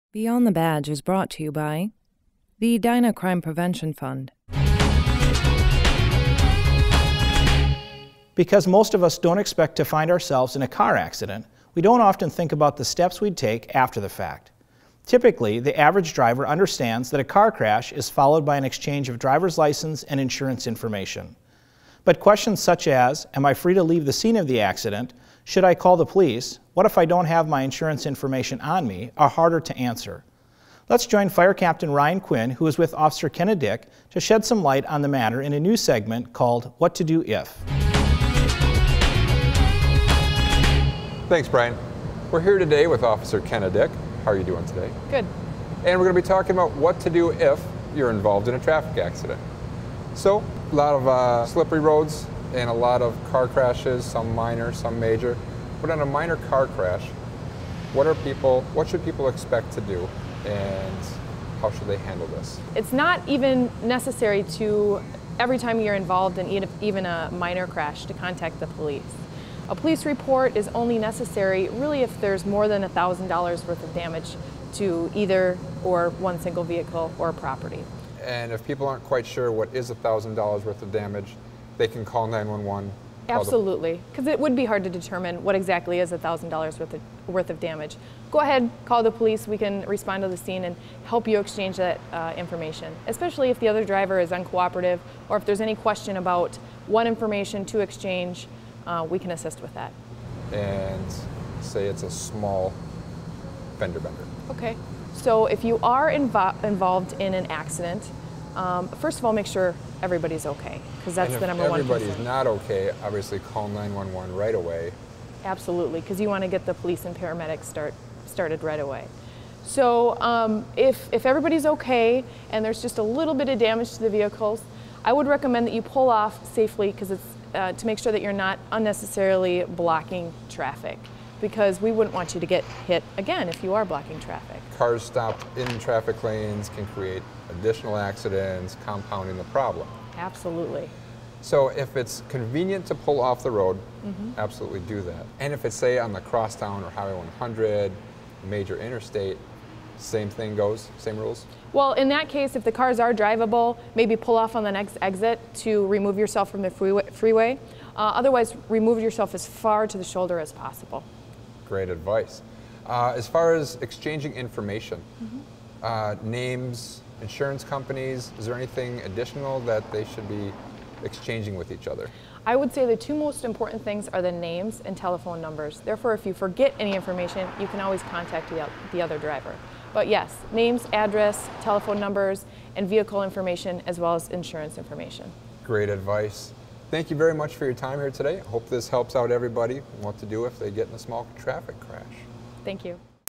Beyond the Badge, February 2014 — 'What To Do If...'
Typically, the average driver understands that a car crash is followed by an exchange of drivers' licenses and insurance information. But questions such as, "Am I free to leave the scene of the accident? Should I call the police? What if I don't have my insurance information on me?" are harder to answer.

Let's join Fire Captain Ryan Quinn who is with Officer Kenna Dick to shed some light on the matter in a new segment called, "What To Do If..."